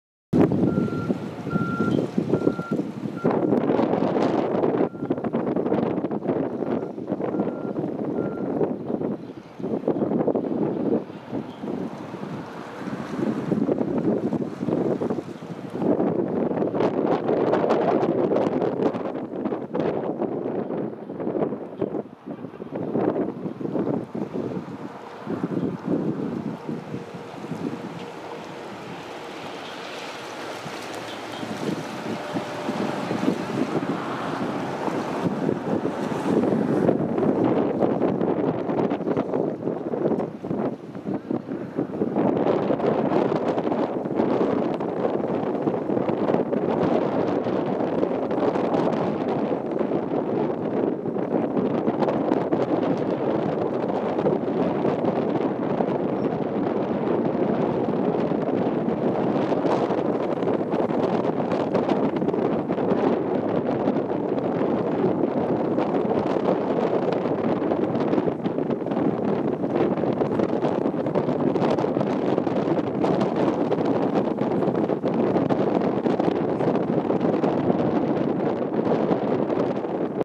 Pato Juarjual - Lophonetta speculatioides
Avistamiento de familia de Patos Juarjuales a orillas del Canal Señoret, Puerto Natales.